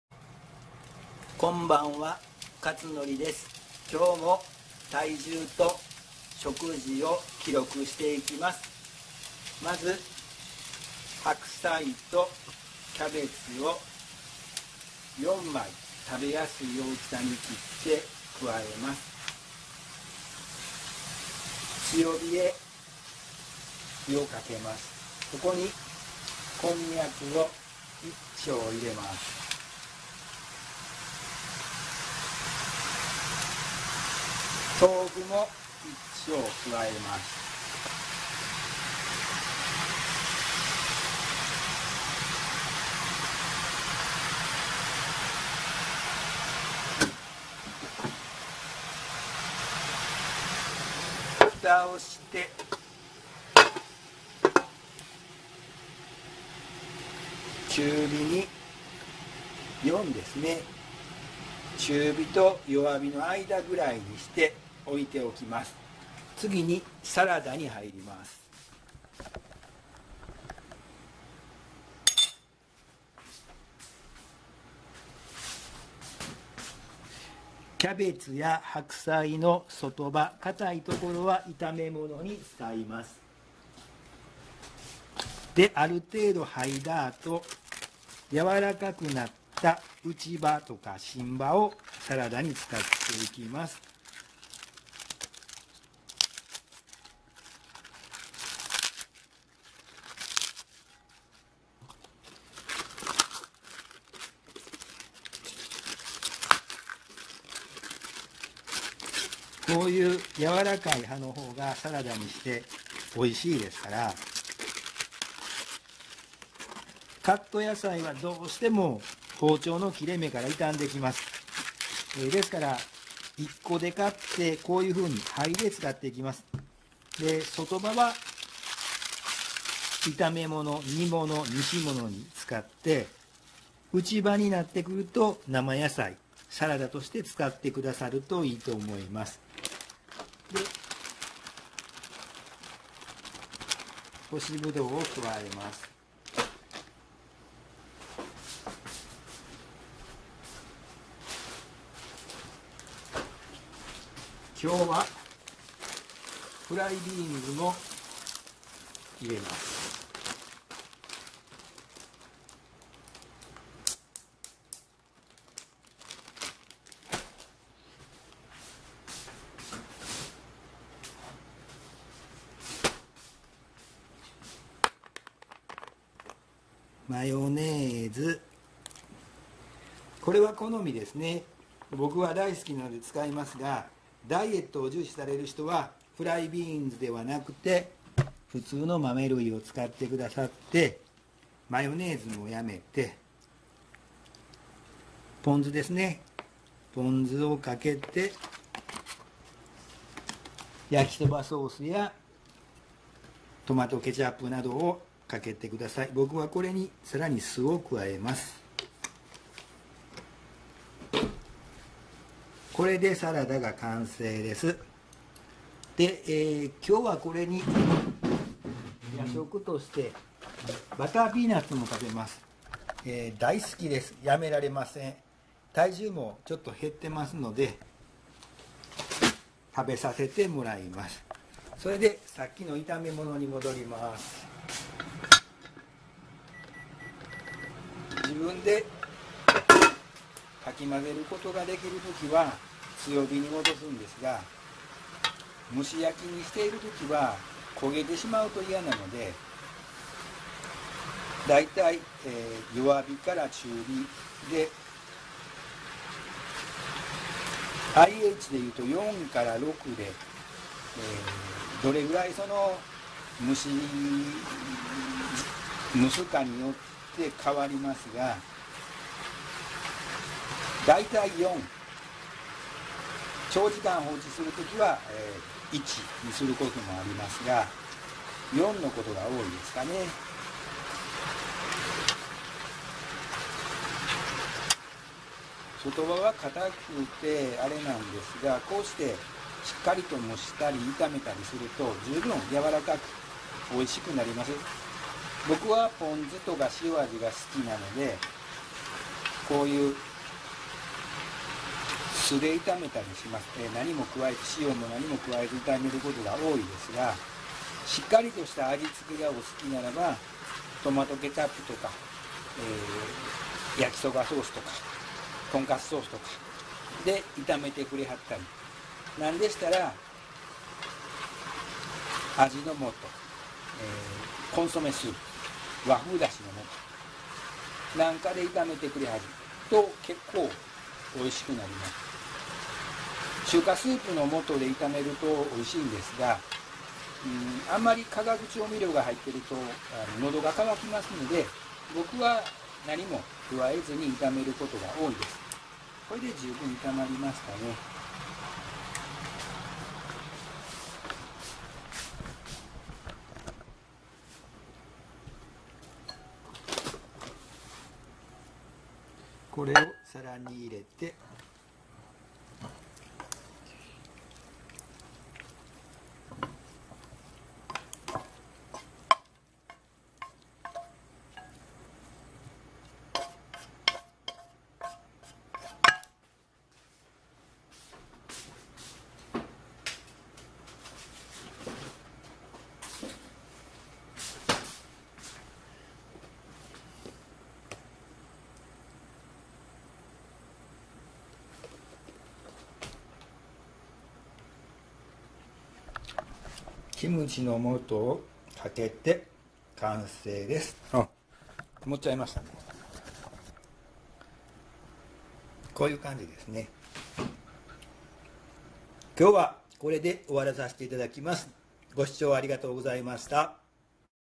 ６月２９日の食事と体重（５９・７ｋｇで４００ｇ減量）
毎日の食事内容と体重の変化を投稿しています。